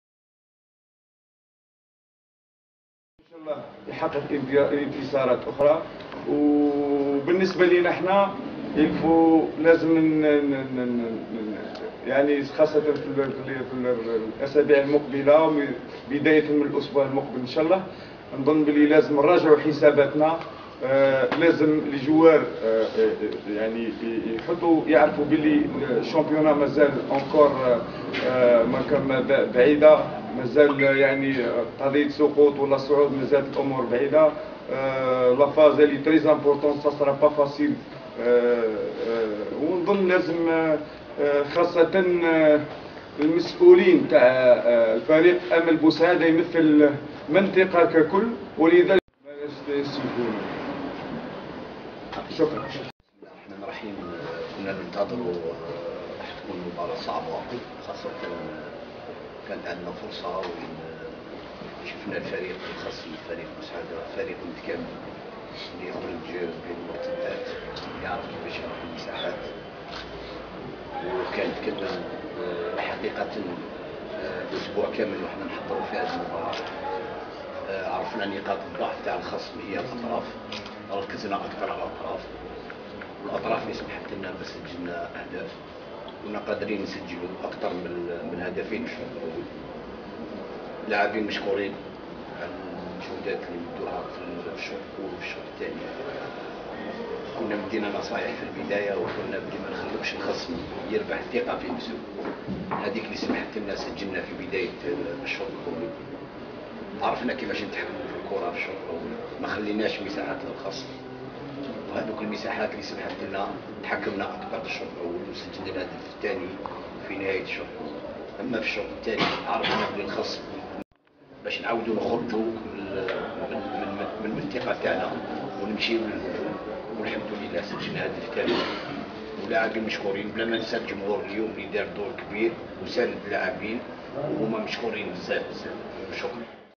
JSMB 3-0 ABS : Déclarations des entraineurs
Les déclarations des entraineurs à la fin de la rencontre qui avait opposé la JSMB à l'A Boussaada.